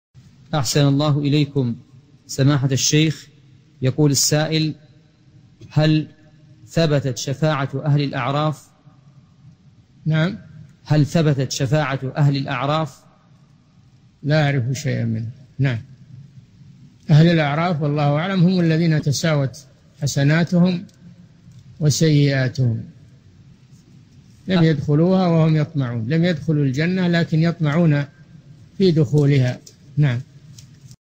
[69 /84] هل ثبتت شفاعة أهل الأعراف؟ | الشيخ صالح الفوزان
[69 /84] السؤال: هل ثبتت شفاعة أهل الأعراف؟
 الجواب: لا أعرف شيئا منها، وأهل الأعراف -والله أعلم- هم الذين تساوت حسناتهم وسيئاتهم {لم يدخلوها وهم يطمعون} [الأعراف : 46] لم يدخلوا الجنة ولكن يطمعون في دخولها."
السيرة الذاتية :
للاشتراك في قناة الشيخ صالح الفوزان
أسئلة وأجوبة من شرح كتاب السنة من سنن أبي داود (84) مقطع

للاستفسار والإقتراحات والدعم
رئيس الفريق
لمشروع كبار العلماء